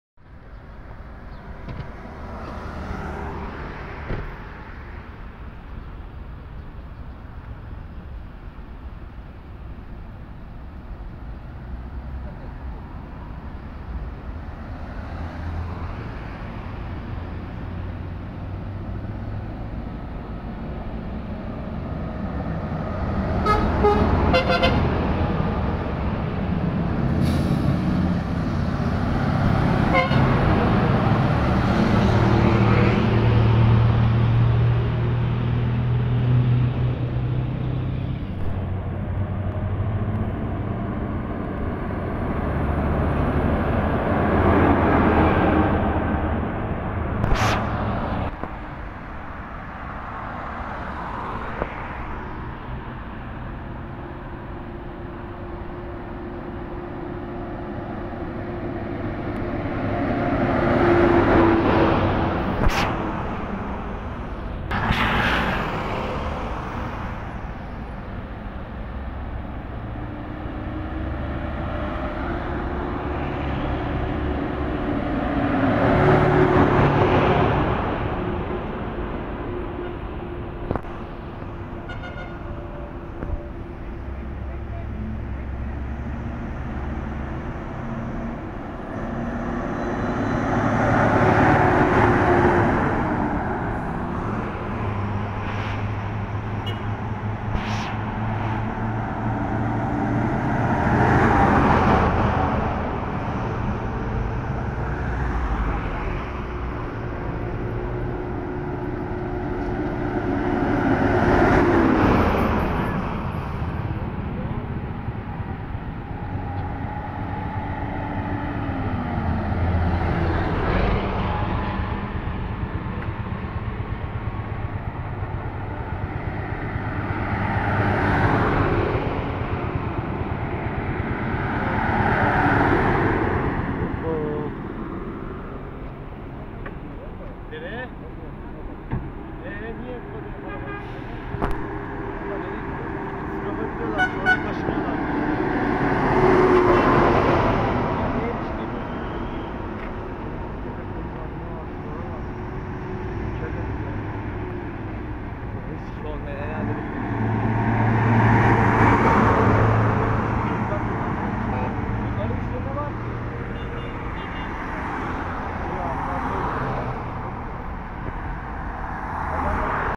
S-400'LER SİNOP'A TEST ATIŞI YAPILMAK ÜZERE SEVK EDİLDİ
Türkiye Cumhuriyeti tarafından Rusya’dan tedarik edilen S-400 hava savunma füze sistemi Bafra’dan geçerek Sinop iline hareket etti.